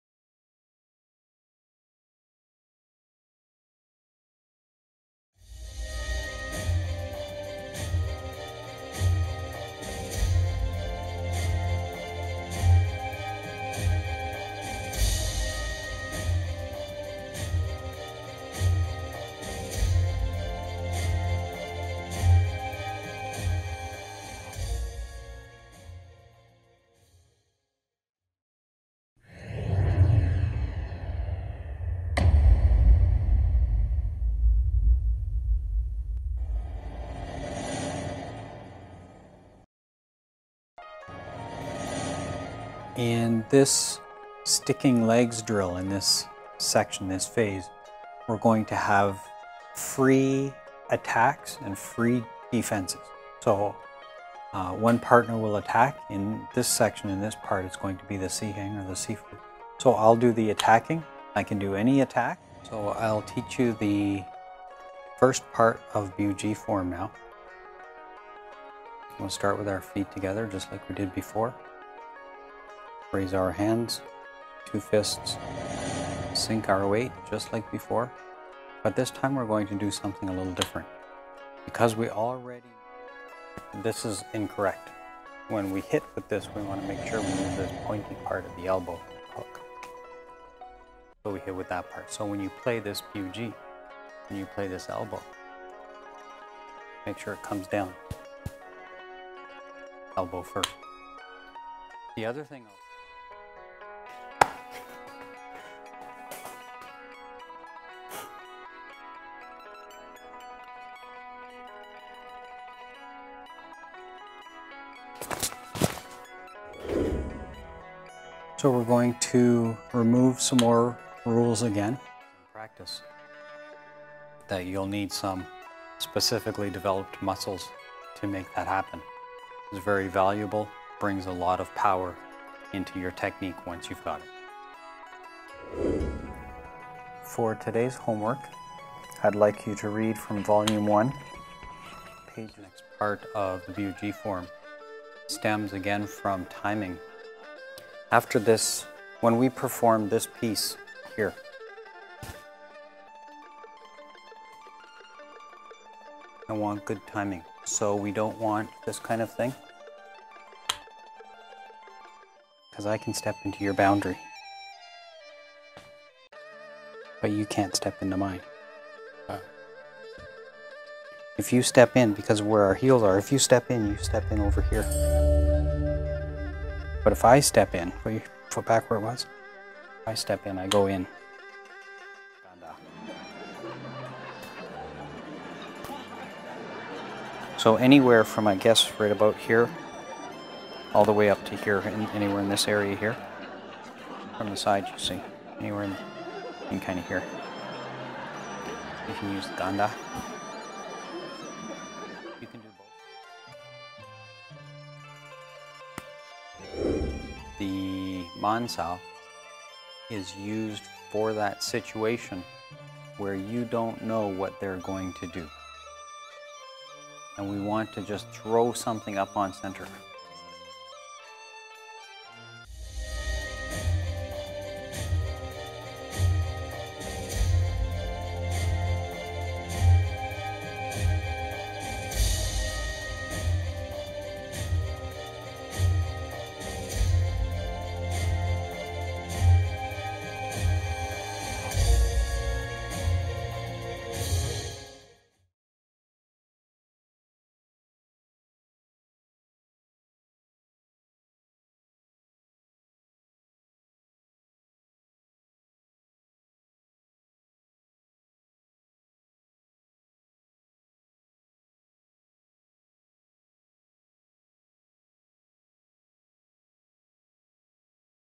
Preview: Wayne Belonoha - Ip Man Wing Chun Blu-Ray 06 - Steps 61-72 of 108 Complete System
This sample is intended to show production quality and/or sample of instruction quality.